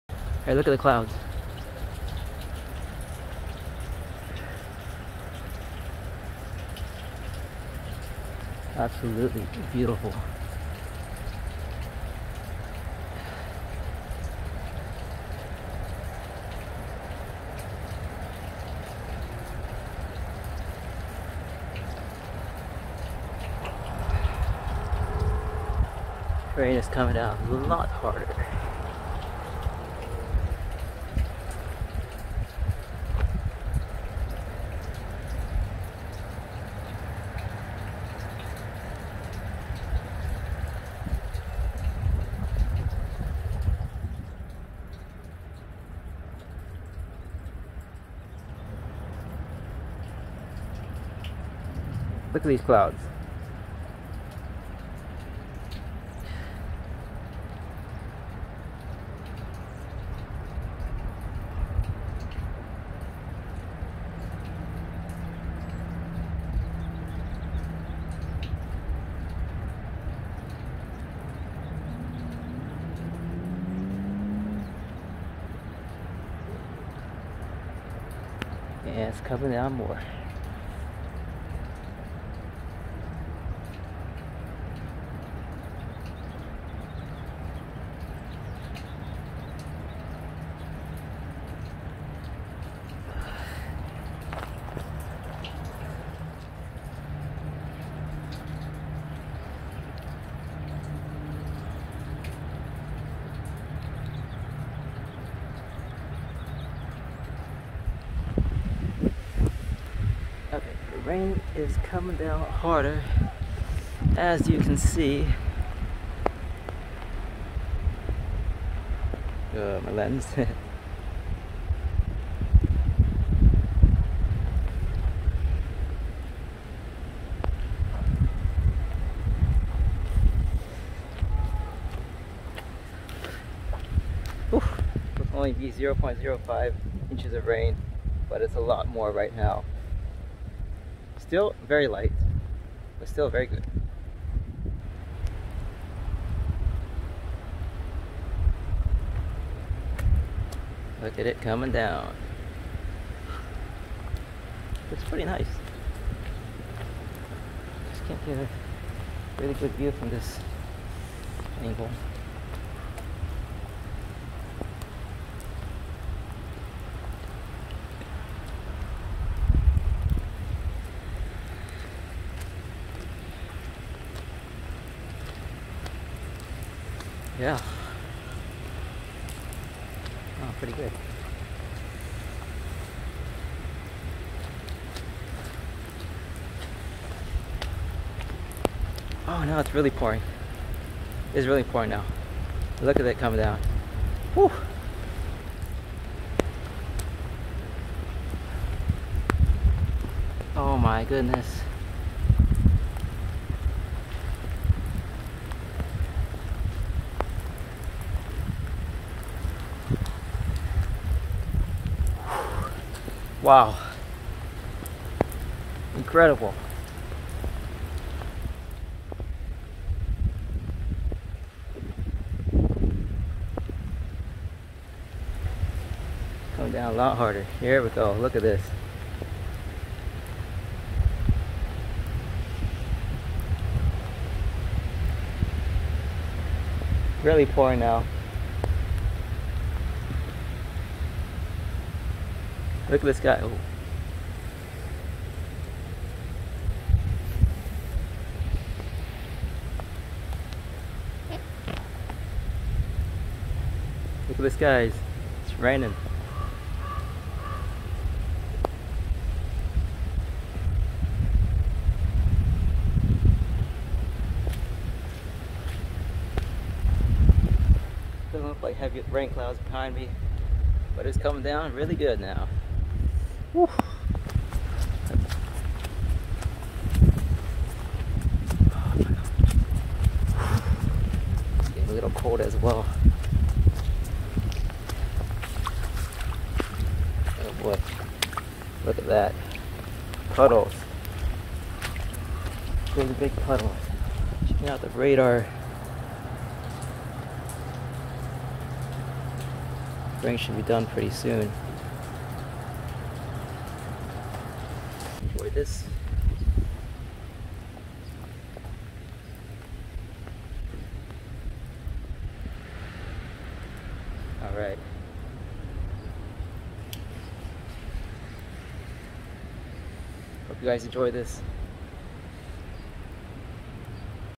Unexpected moderate rain chase with amazing nimbostratus clouds!!!
Did not expect much from this rain chase, however ended up with decent moderate rain!